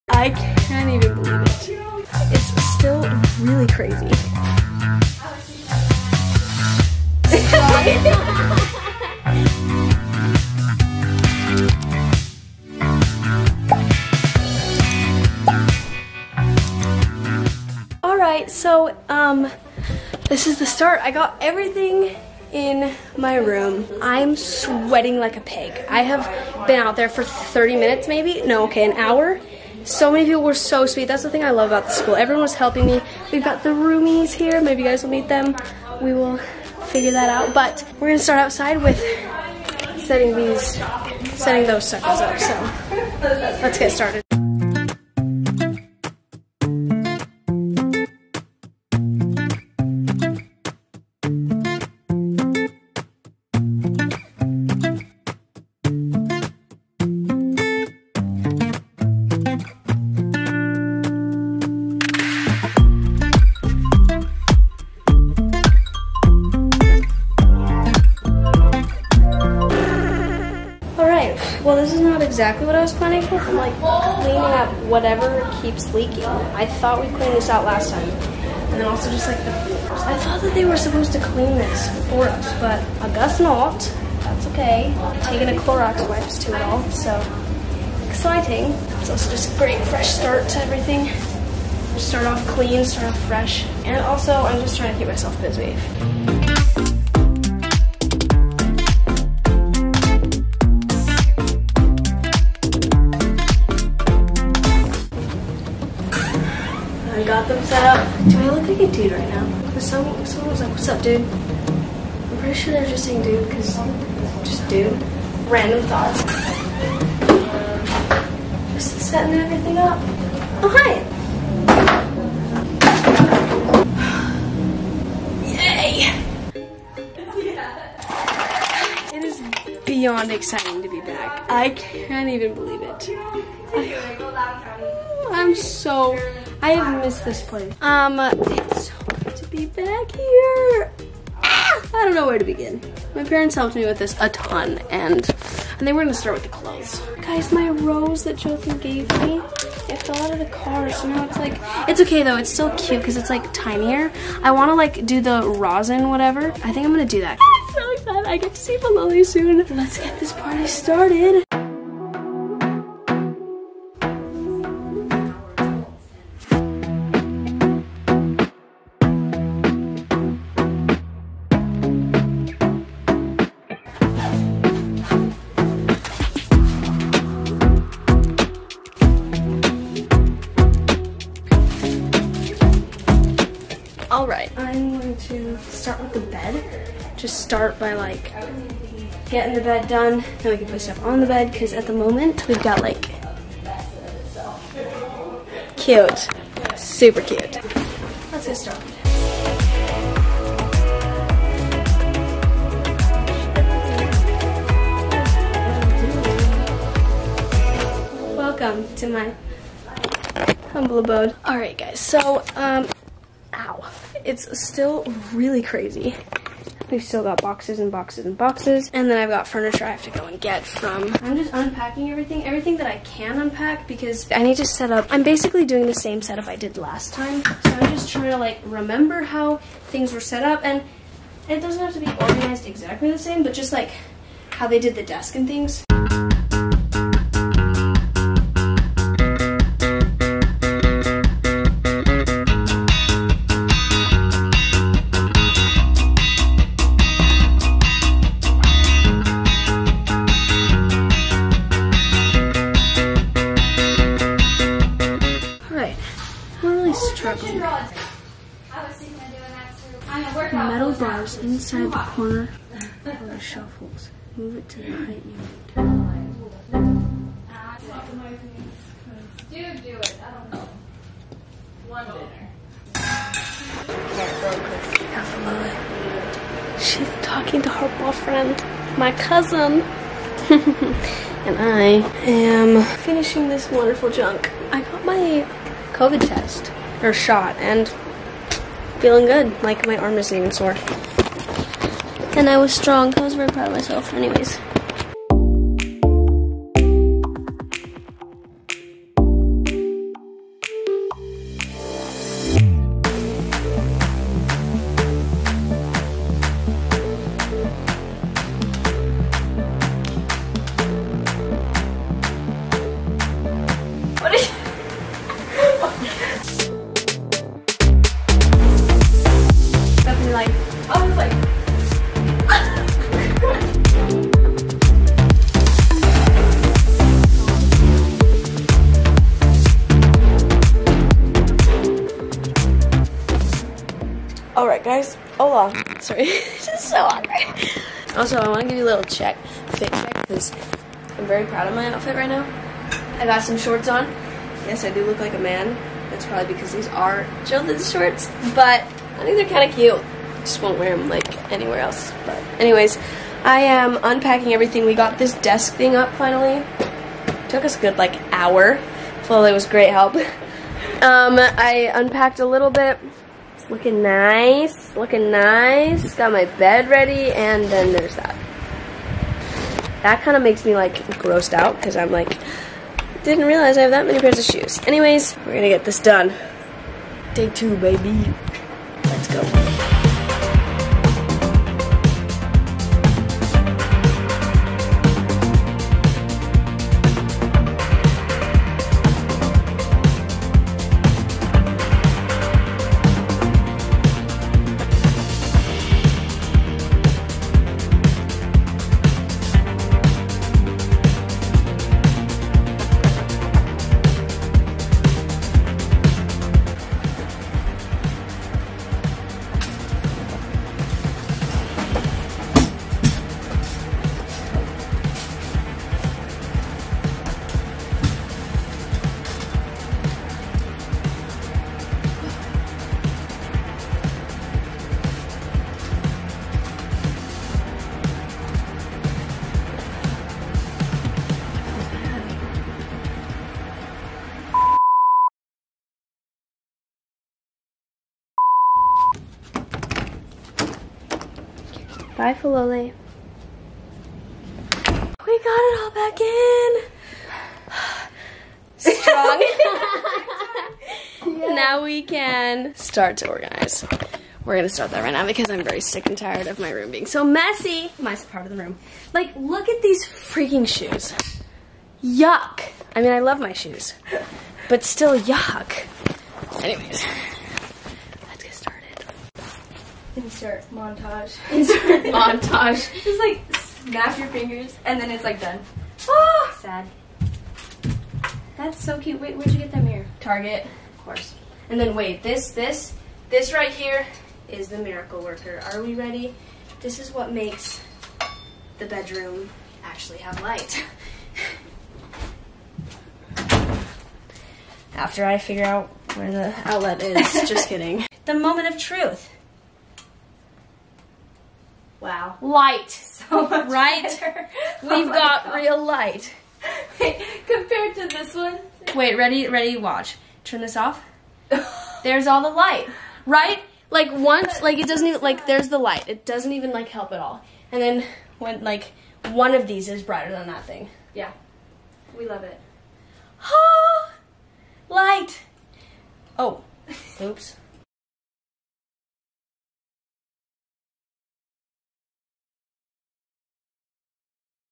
unpacking and moving BACK into my apartment!! *all alone*
and we are back!! I can't wait for this new semester here at college, and I can't wait for you guys to see all the adventures!!

250 N Red Cliffs Dr. 4B No.366 Saint George UT, 84790